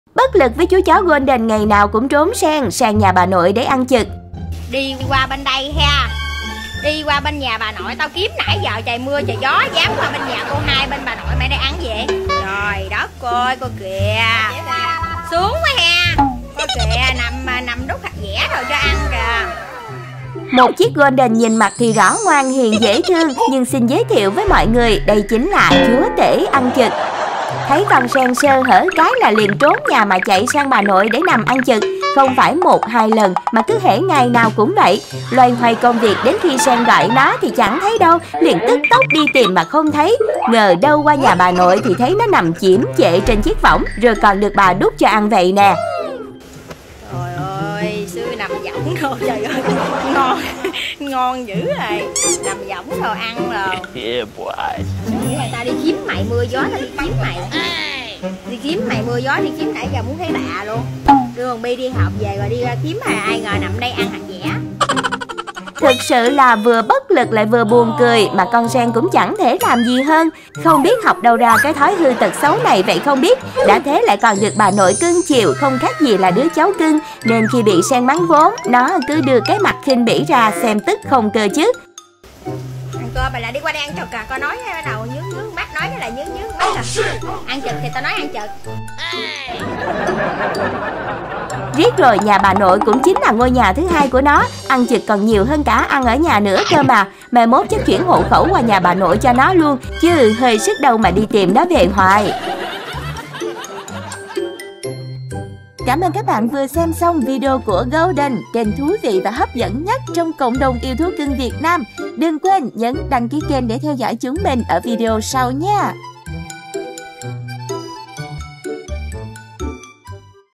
Bất lực với 'CHÚ CHÓ GOLDEN' ngày nào cũng trốn Sen sang nhà Bà Nội để 'ĂN CHỰC' 😂 | Gâu Đần
🐶Gâu Đần🐶